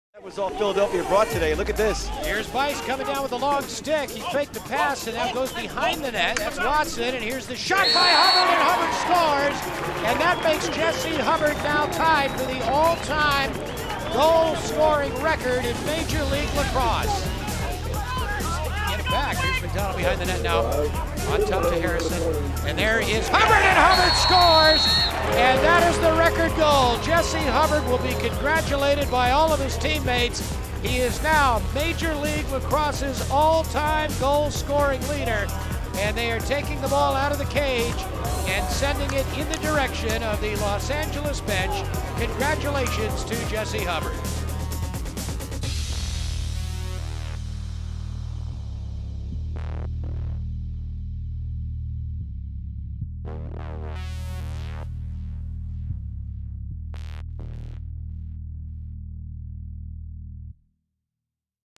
Hall of Fame Moments: Jesse Hubbard MLL Scoring Record
On July 26th 2008 Jesse Hubbard surpassed Mark Millon to become the All-Time leading goal scorer in Major League Lacrosse history.*

Hubbard was one of the original 40 members of the league playing in the Summer Showcase in 2000. He was a six-time all-star in Major League Lacrosse (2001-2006) and the MLL's leading scorer three times (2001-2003). Hubbard was a pioneer for the league and was instrumental in marketing the league looking to showcase premium talent. He was named to the MLL 10-Year Anniversary Team.

Hubbard is one of eight lacrosse legends being inducted in to the National Lacrosse Hall of Fame Saturday October 20th 2012. Joining him are fellow MLL All-Stars Brian Dougherty and Roy Colsey.

Tim Goettelmann holds the current All-Time MLL Goals record with 268 Career Goals. A record which he broke in 2010.